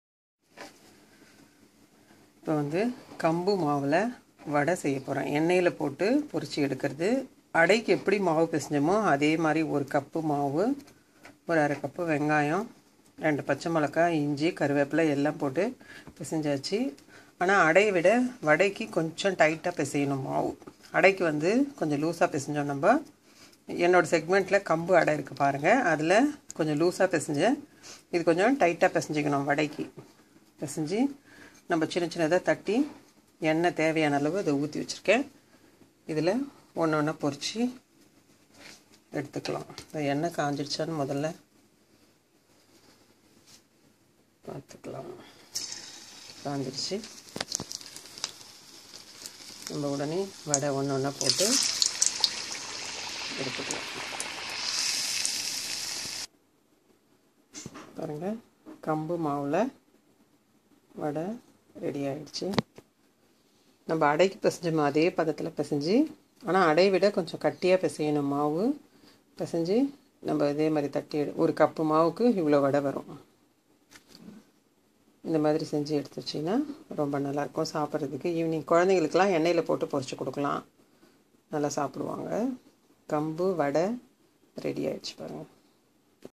Kambu Vadai | கம்பு வடை | Spicy Pearl Millet Vada [Fritters, Flafel]
Pearl Millet Flour / கம்பு மாவு - 1 Cup
Onion / வெங்காயம் - 1
Green Chillies / பச்சை மிளகாய் - 3
Ginger / இஞ்சி - 1 inch piece
Curry Leaves / கறிவேப்பிலை - As required
Water / நீர் - As Required
Salt / உப்பு - As Required